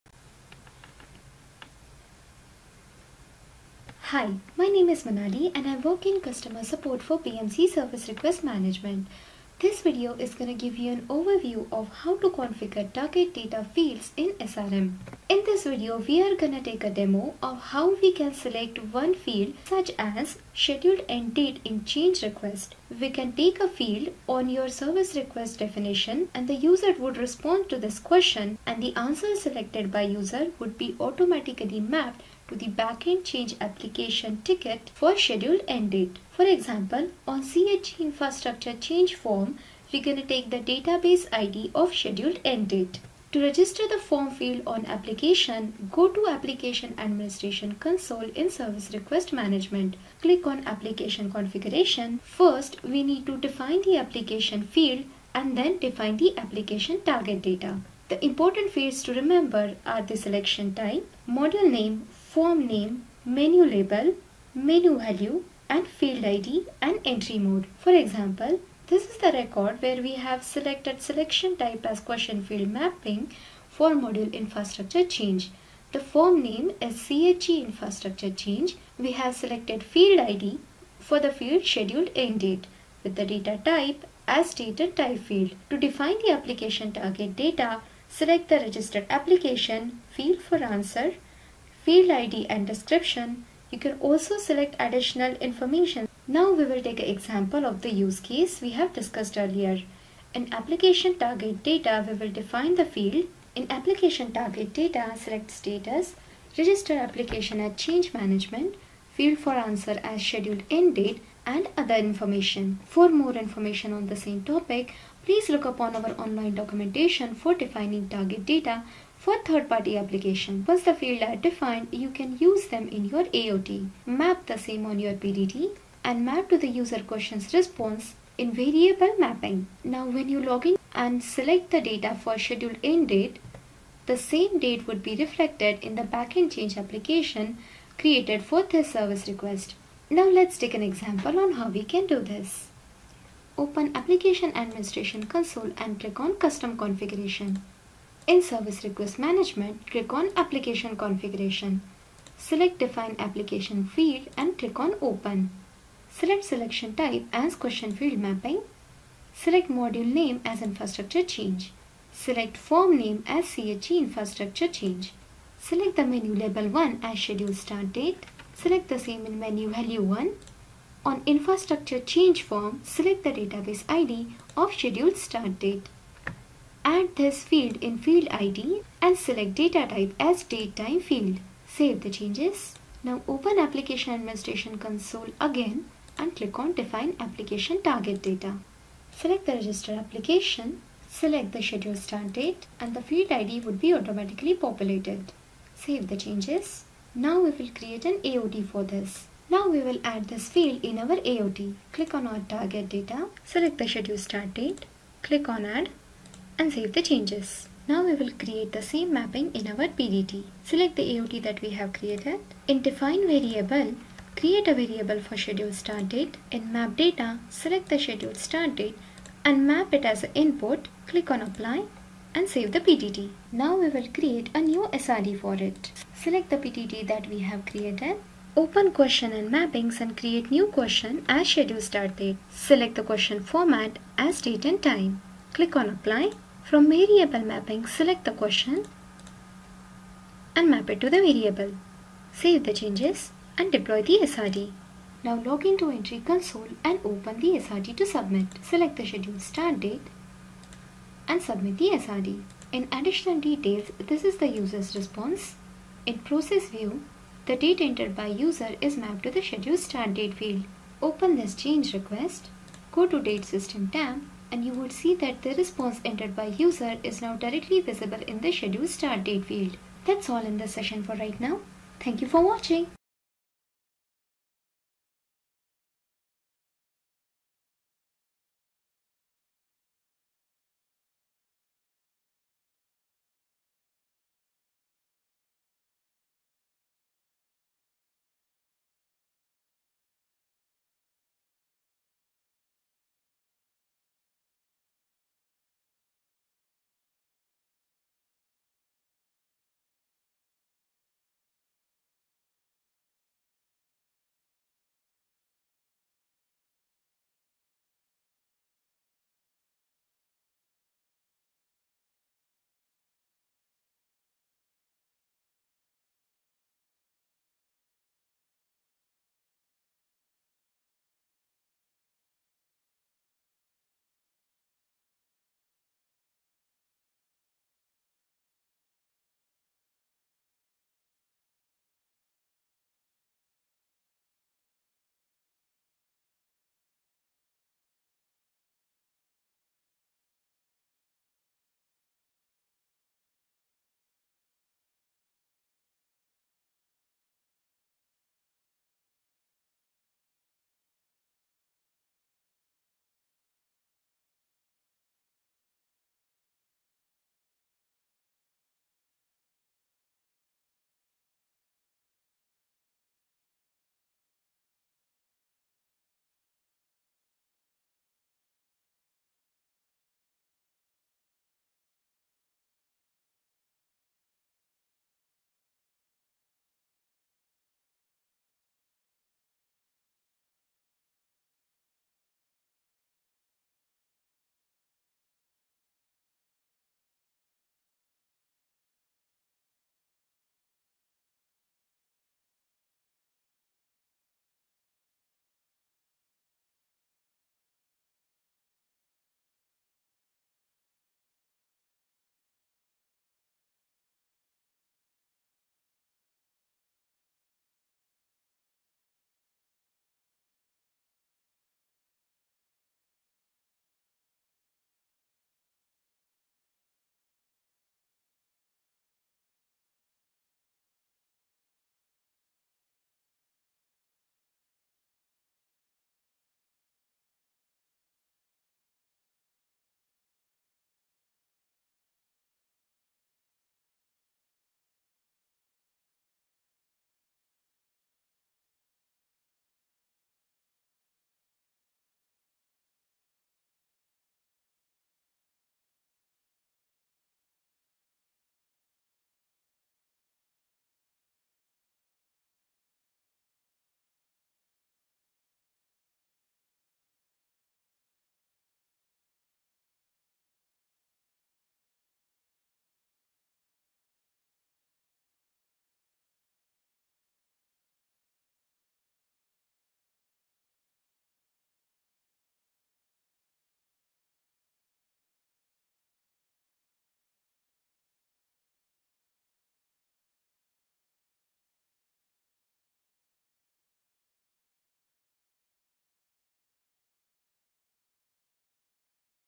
BMC Service Request Management 9.1 - Configuring target data fields in Service Request Management
Overview of how to configure target data fields in Service Request Management. For more information on this topic, see the BMC Online Documentation Portal